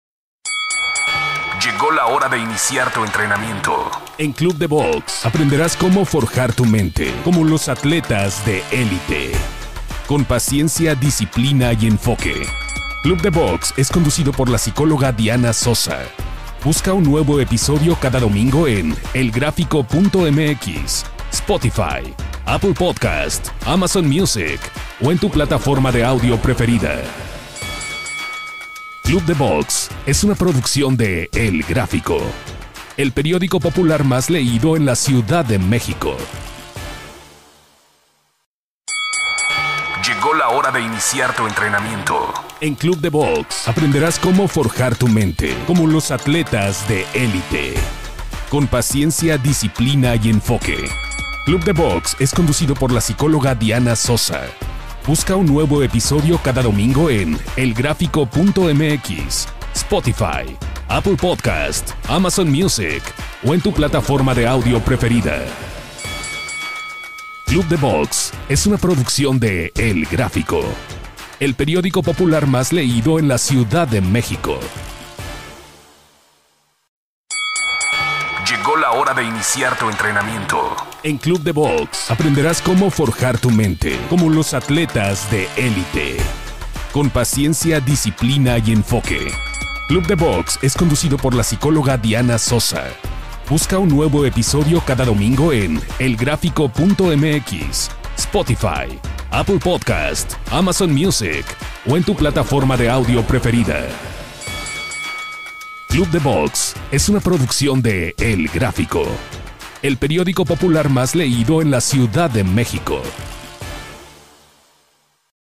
BIENVENIDOS A CLUB DE BOX
LLEGÓ LA HORA DE INICIAR TU ENTRENAMIENTO EN CLUB DE BOX APRENDERÁS CÓMO FORJAR TU MENTE COMO LOS ATLETAS DE ÉLITE CON PACIENCIA, DISCIPLINA Y ENFOQUE.

CLUB DE BOX ES CONDUCIDO POR LA PSICÓLOGA DIANA SOSA.

BUSCA UN NUEVO EPISODIO CADA DOMINGO.

CLUB DE BOX ES UNA PRODUCCIÓN DE EL GRÁFICO
EL PERIÓDICO POPULAR MÁS LEÍDO EN LA CIUDAD DE MÉXICO.